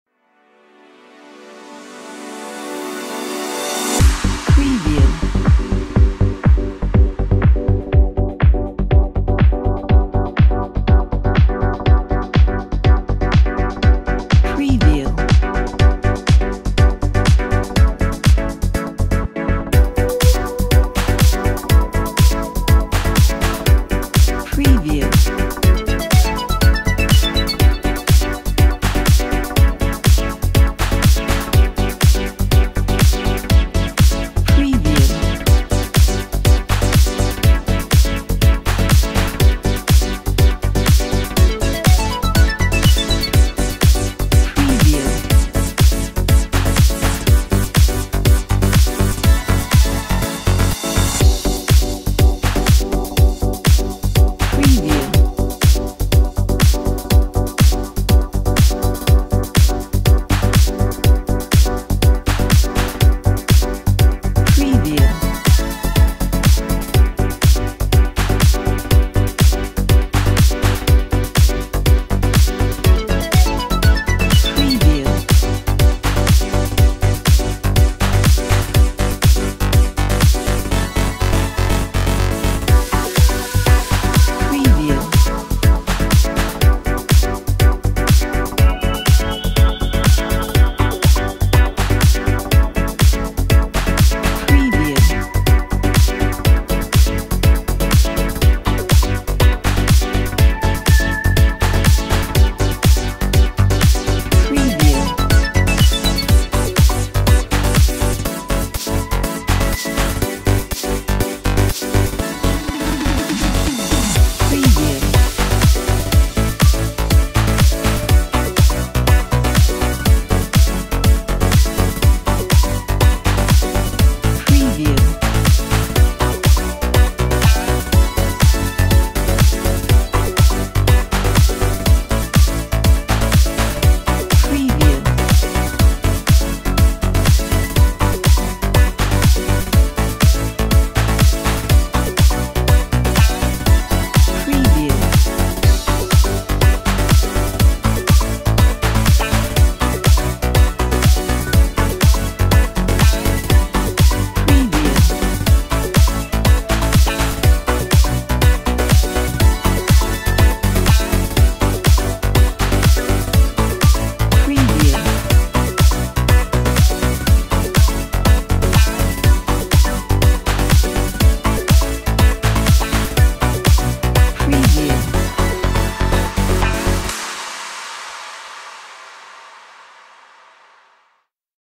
House Background Royalty Free Stock Music
This is one a positive, stylish, optimistic, flying house track with an uplifting groove, easy mood and, modern synths.
Used for advertising, time lapses, youth auditory, vlogs, fashion and more.